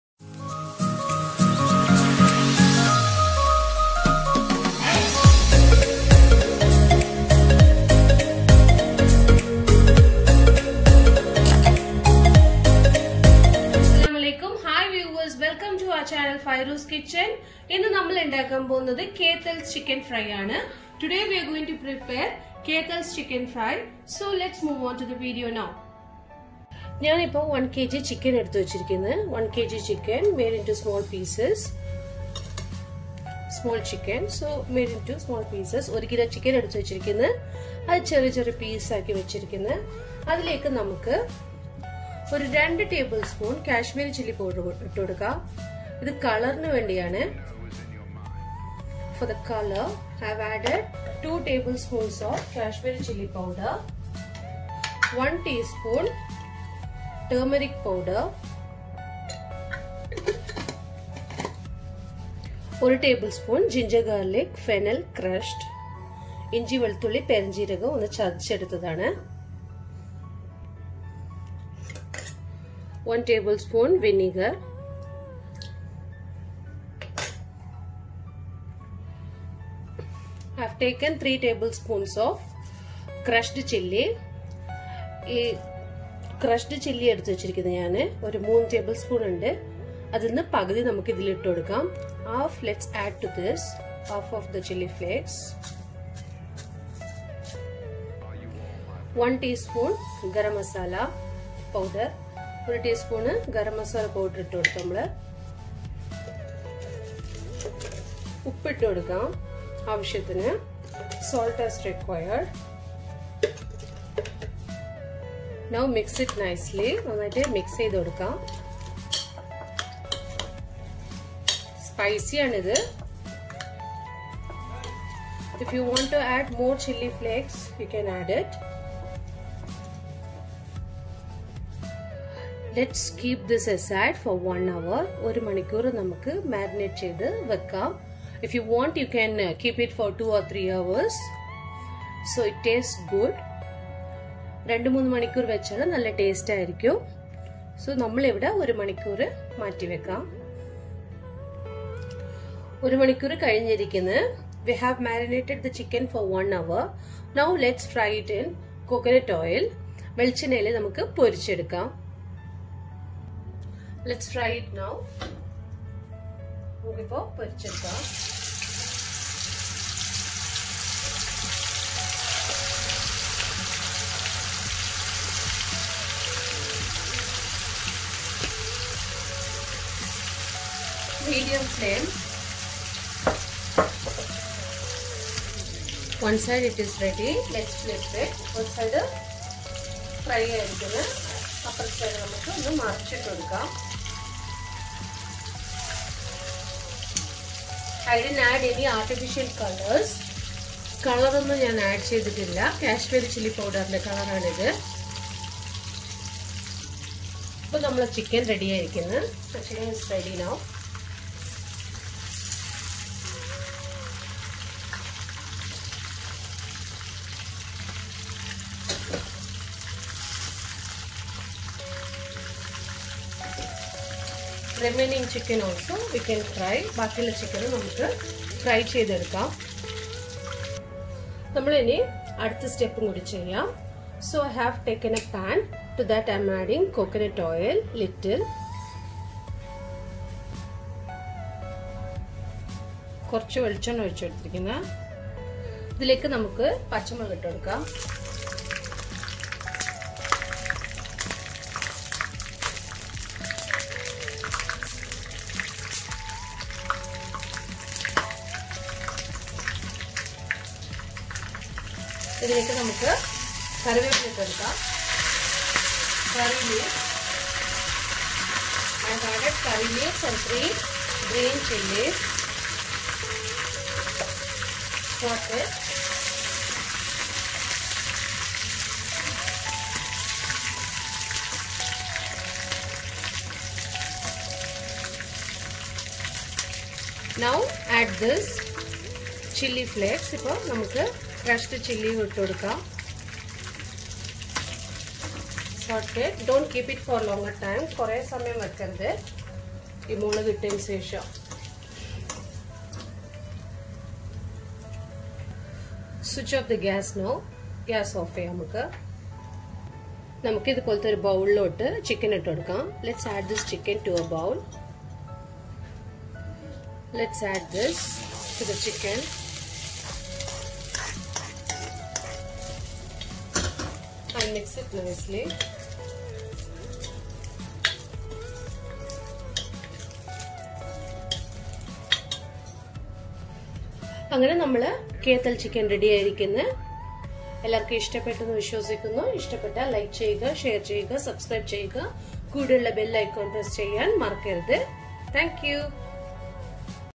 Kethal's chicken 🐔 fry/ tasty and spicy/ no artificial colours/ pls try
Ingredients:
Chicken- 1 kg
Kashmiri chilli powder- 2tbsp
Turmeric powder-1 tsp
Ginger garlic fennel seeds crushed- 1 tbsp
Apple cider Vinegar- 1tbsp
Crushed chilli- 3tbsp
Garam masala powder-1 tsp
Salt
Curry leaves
Coconut oil